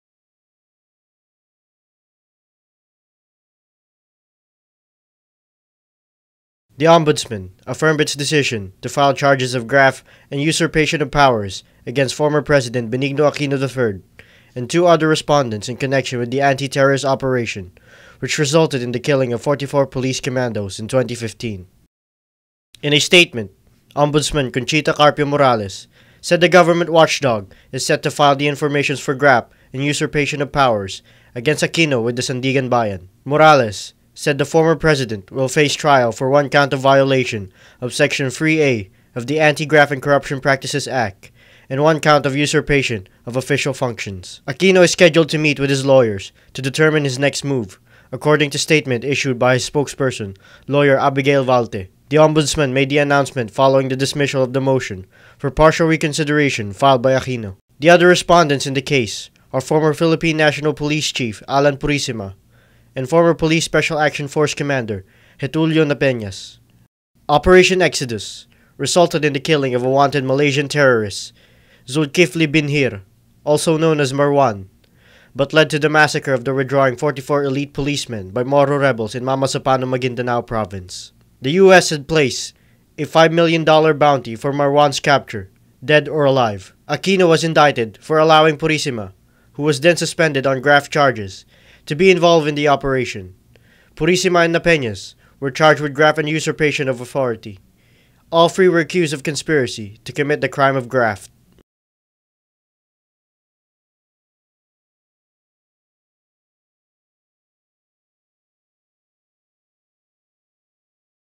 Ombudsman affirms decision to file charges vs Aquino
Ombudsman affirms its decision to file charges vs former President Benigno Aquino III
Report by: Celerina Monte
Text: Virgilio Galvez
Video: Christian Dimitui
Executive Producer: Toshihiko Uriu
Executive Editor / Head, News Video Production: Virgilio Galvez
Supervising Producer: Onin Tagaro

- manila shimbuntv